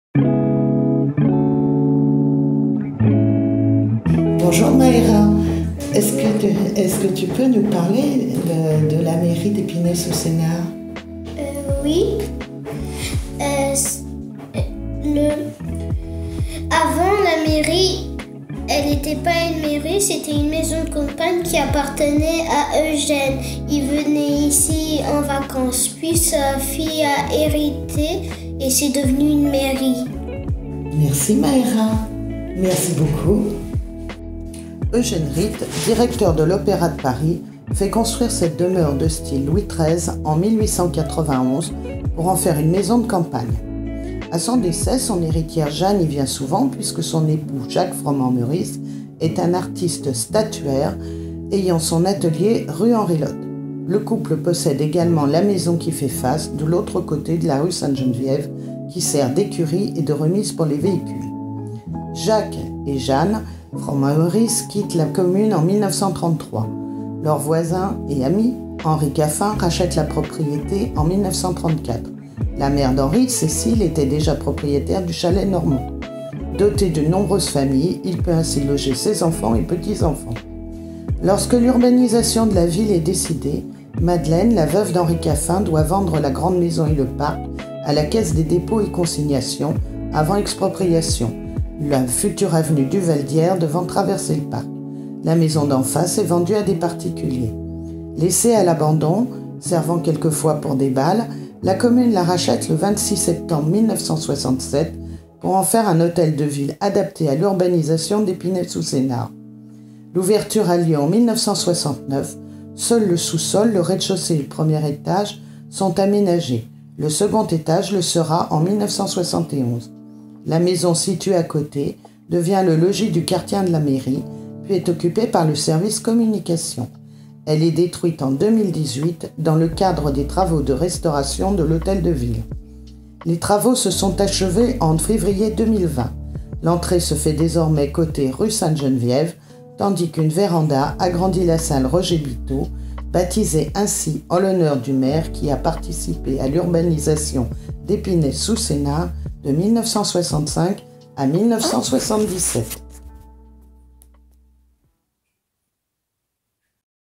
L'Hôtel de Ville d'Épinay-sous-Sénart
Découvrez l'histoire d'un des plus imposants bâtiments, l'actuel Hôtel de Ville d'Épinay-sous-Sénart.